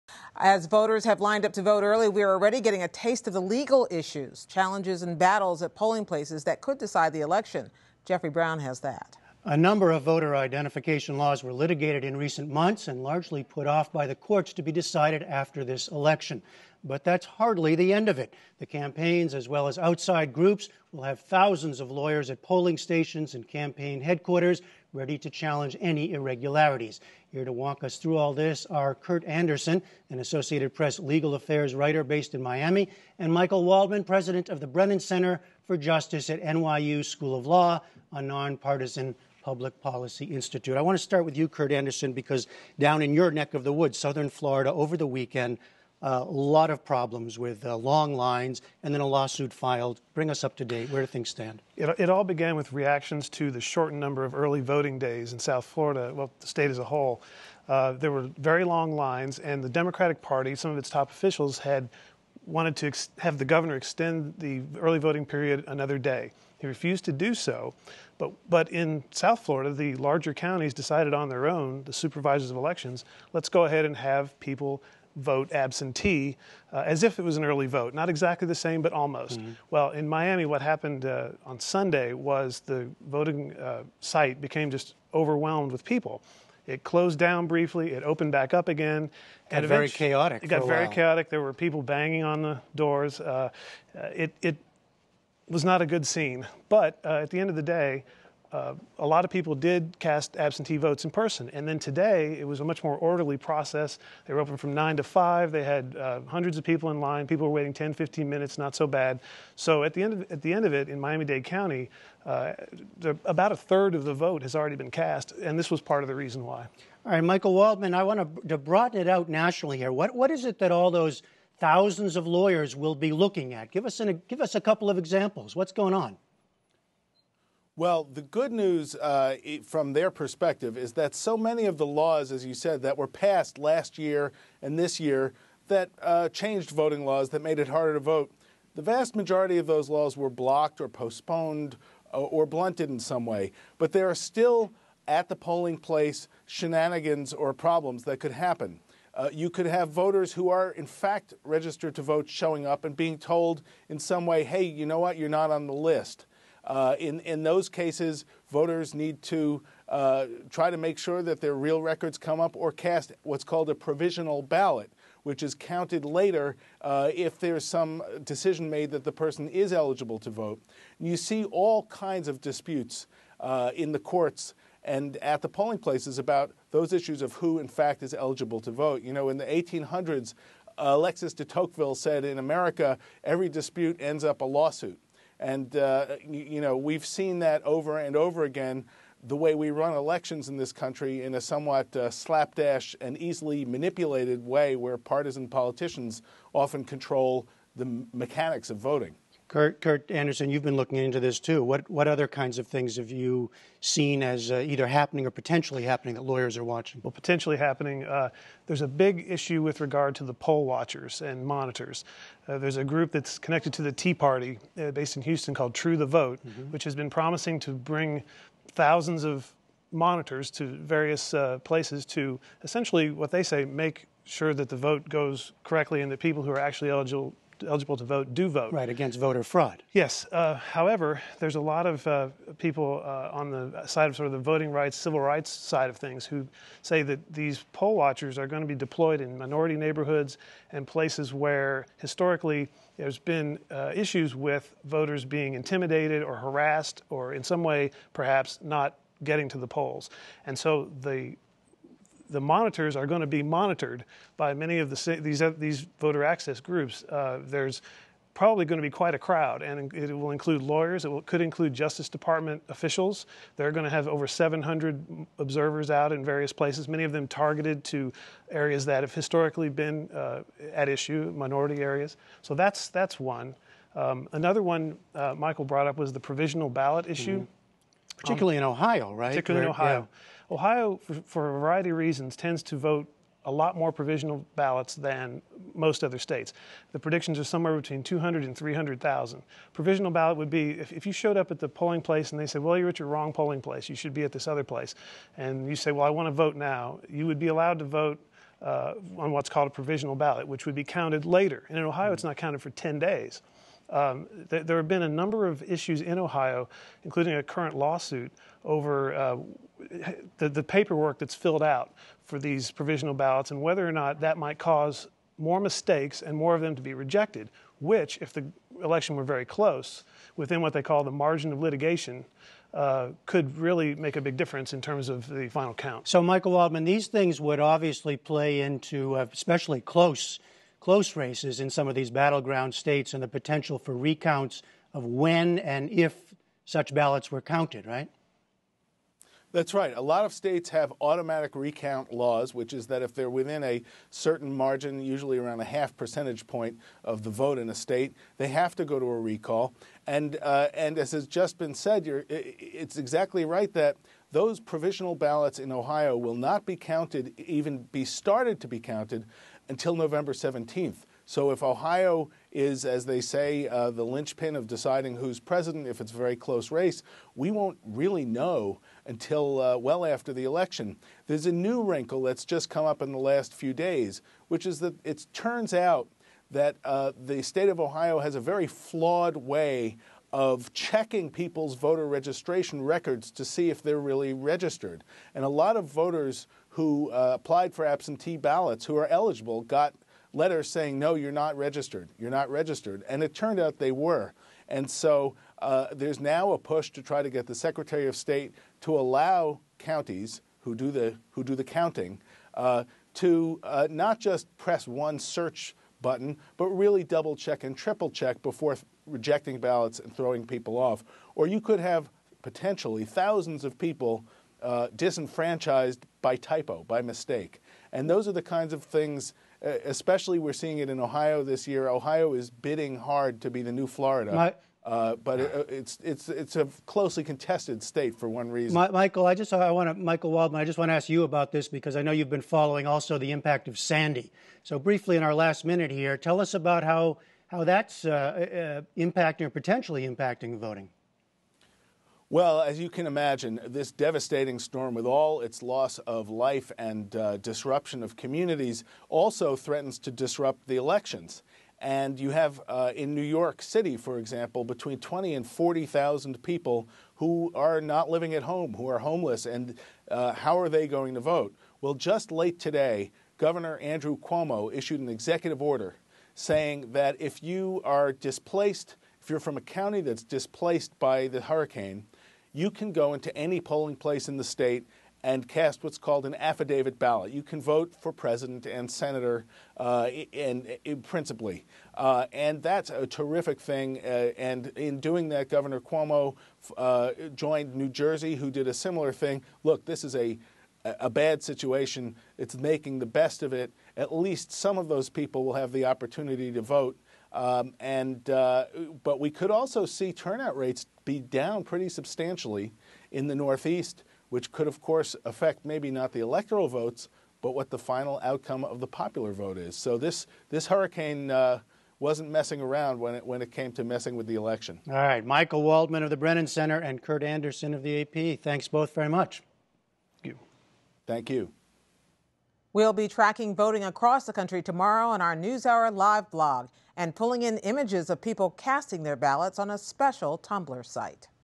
How Will Controversial Voter I.D. Laws Affect Poll Turnout?
Lawyers gear up to monitor polling stations on Election Day to ensure voter fraud is limited and voter rights are protected. Jeffrey Brown talks to The Associated Press' Curt Anderson and Brennan Center for Justice's Michael Waldman on what to expect, especially in light of legal battles on early voting and new voter I.D. laws.